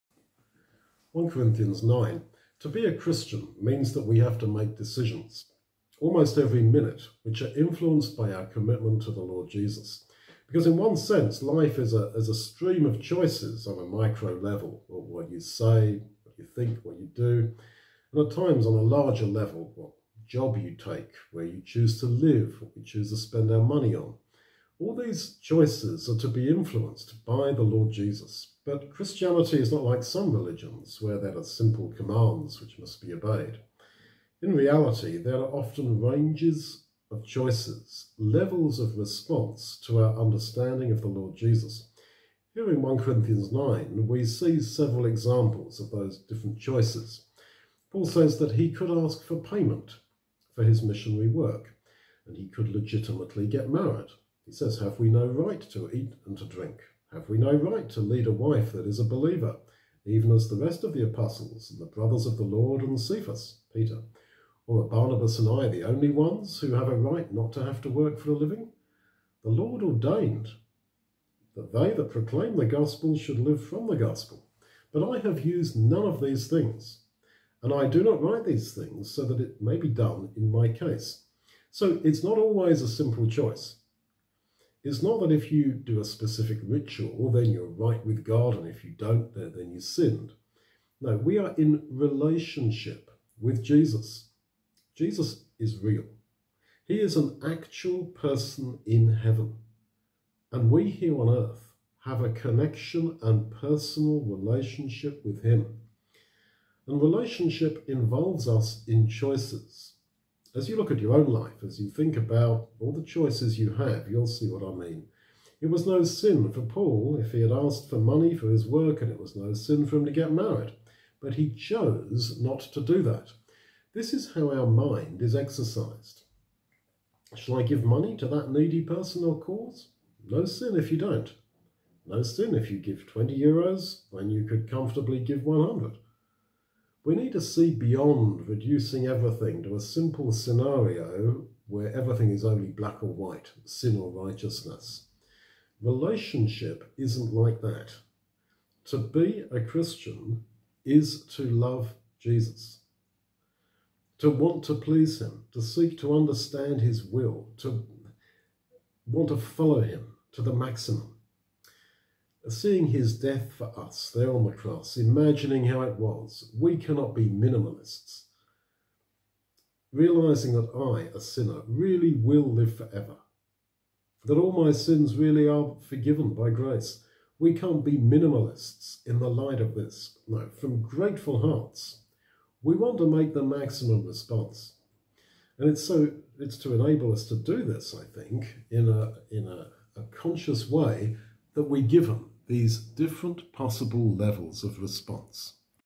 1 Corinthians 9 Differing levels of response to Christ
1 CORINTHIANS 9
To be a Christian means that we have to make decisions, almost every minute, which are influenced by our commitment to the Lord Jesus. Because in one sense, life is a stream of choices, on a micro level (what we think about, what we say), and at times on a larger level (what job we take, where we choose to live, what we spend our money on). All these choices are to be influenced by the Lord Jesus. But Christianity is not like some religions, where there are simple commands which must be obeyed. In reality, there are often ranges of choices, levels of response to our understanding of the Lord Jesus.

Here in 1 Corinthians 9, we see several examples of these choices. Paul says that he could ask for payment for his missionary work; and he could legitimately get married: “Have we no right to eat and to drink? Have we no right to lead a wife that is a believer, even as the rest of the apostles and the brothers of the Lord and Cephas? Or are Barnabas and I the only ones who have a right not to have to work for a living?... the Lord ordained that they that proclaim the gospel should live from the gospel. But I have used none of these things, and I do not write these things that it may be so done in my case” (:4-6,14,15).

So it’s not always a simple choice; it’s not that if you do a specific ritual, then you are right with God. If you don’t, then you sinned. We are in relationship with Jesus. Jesus is real. An actual person in Heaven, and we here on earth have a connection and personal relationship with Him. And relationship involves us in choices. As you look at your own life, as you think about all the choices you have, you will see what I mean. It was no sin for Paul to have asked for money for his work, and it was no sin for him to get married. But he chose not to do that.

This is how our mind is exercised. Shall I give money to that needy person or cause? No sin if you don’t. No sin if you give 20 Euros when you could comfortably give 100 Euros. We need to see beyond reducing everything to a simple scenario where everything is only black or white, sin or righteousness. Relationship isn’t like that. To be a Christian is to love Jesus, to want to please Him, to seek to understand His will, His principles- and to follow them to the maximum. Seeing His death for us, imagining how it was… we cannot be minimalists. Realizing that I, a sinner, really will live for ever, that all my sins are really forgiven… we cannot be minimalists in the light of this. From grateful hearts, we want to make the maximum response. And it’s to enable us to do this, that we have these different levels of possible response.
Best free Bible app: Text, commentary on every Bible verse, three audio files for every chapter: text, message and youth message. No ads, no popups. For Android
 Carelinks Ministries